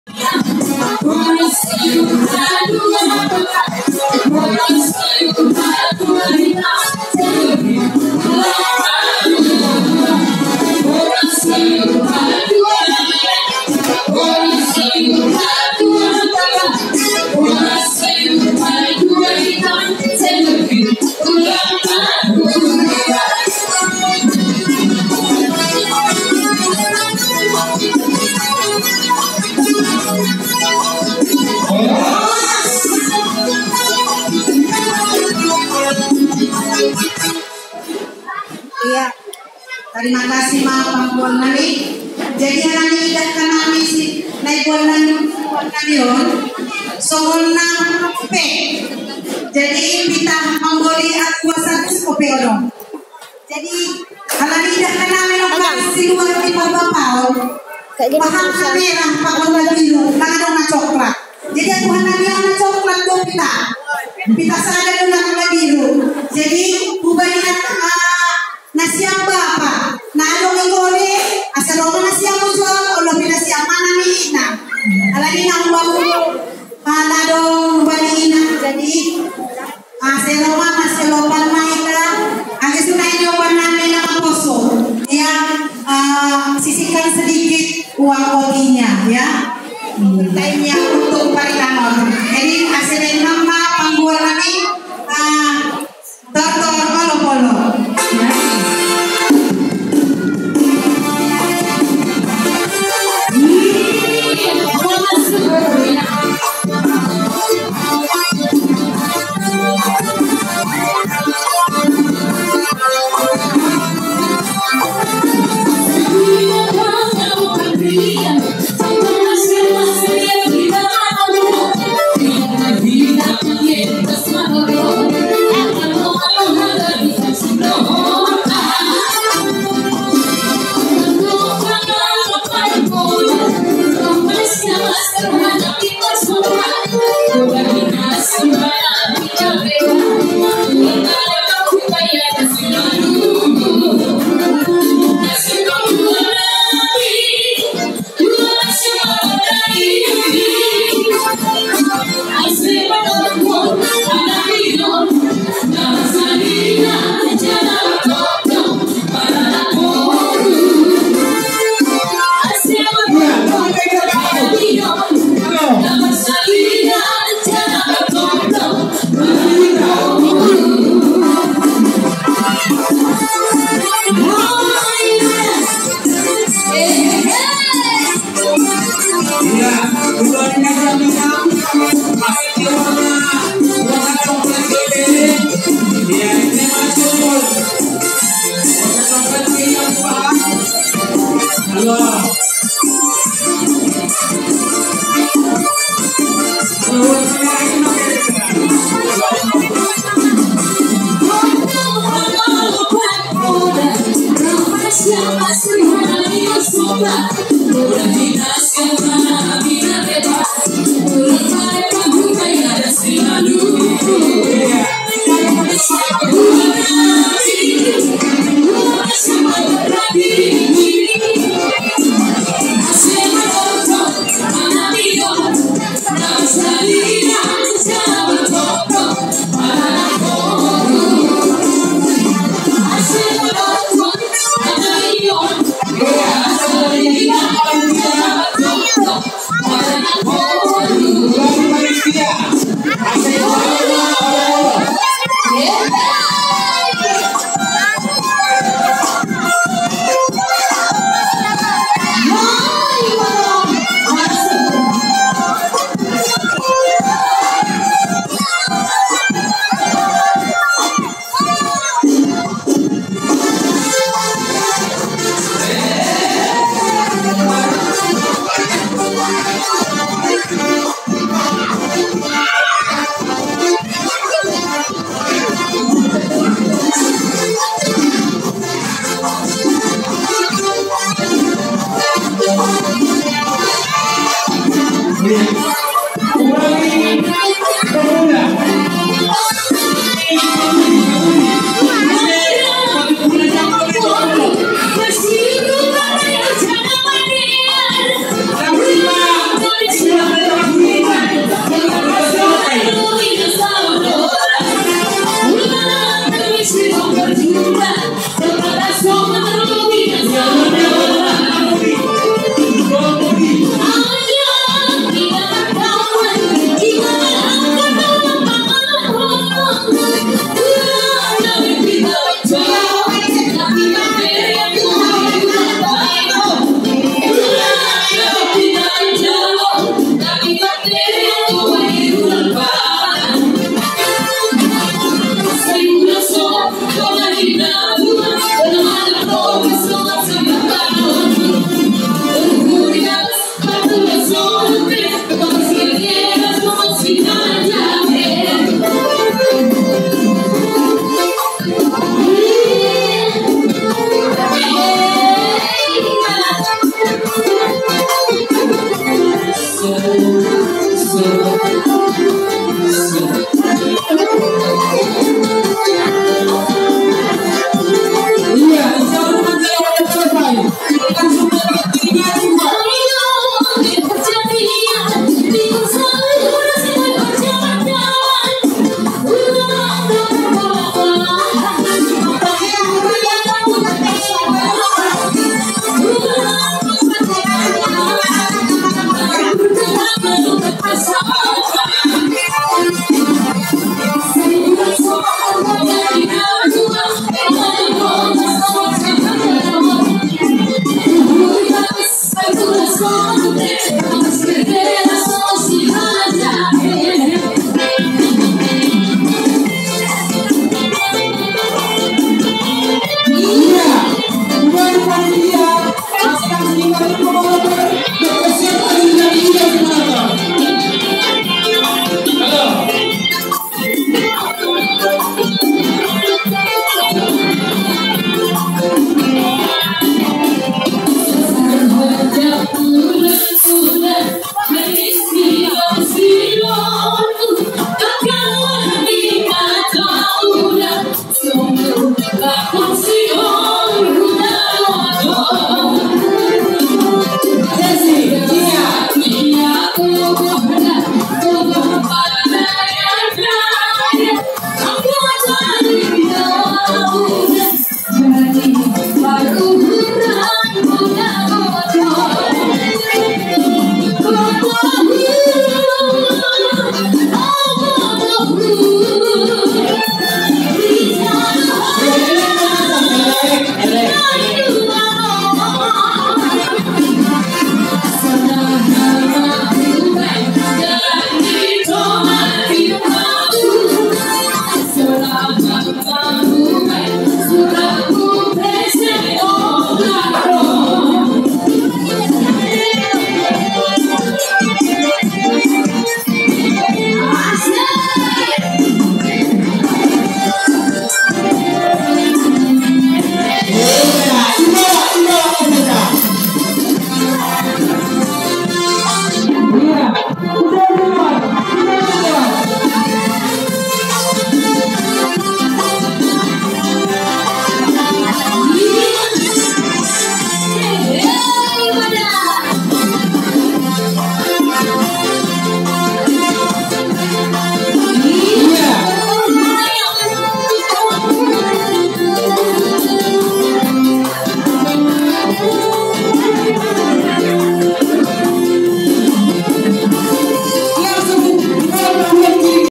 Tortor simalungun
Pesta inang GKPS ke 62 , GKPS BATUAJI BATAM BARAT